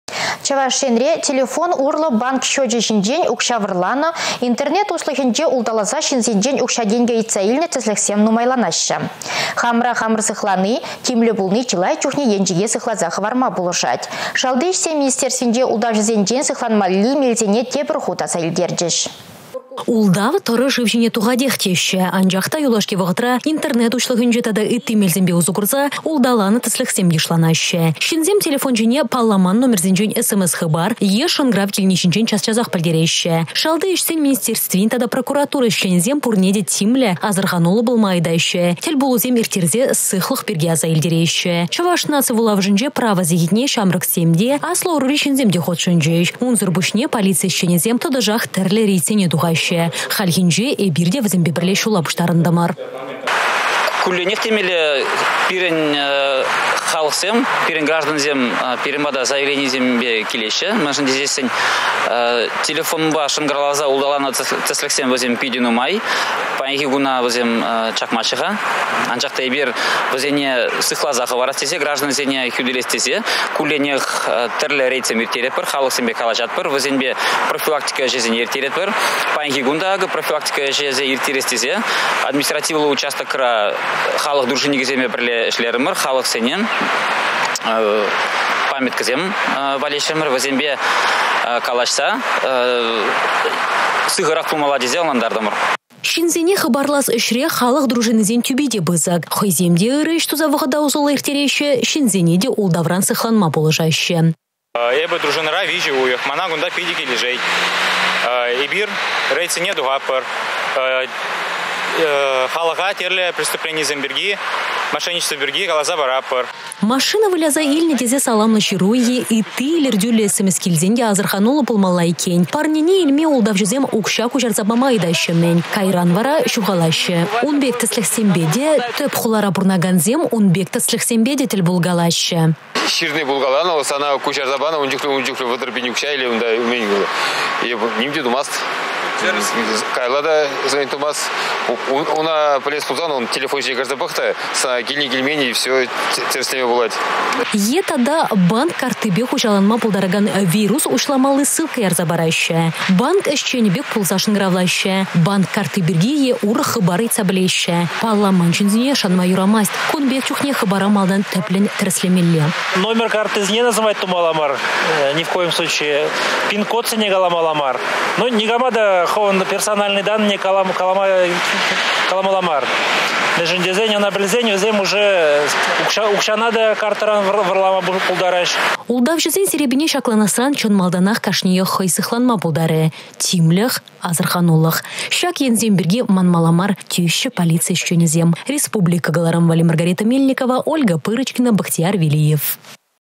Асăрханăр, ултавçăсем!
Чăваш Енре телефон урлă банк счечĕ çинчен укçа вăрланă, интернет уçлăхĕнче улталаса çынсенчен укçа-тенкĕ ыйтса илнĕ тĕслĕхсем нумайланаççĕ. Хамăра хамăр сыхлани, тимлĕ пулни чылай чухне енчĕке сыхласа хăварма пулăшать. Шалти ĕçсен министерствинче ултавçăсенчен сыхланмалли мелсене тепĕр хут аса илтерчĕç.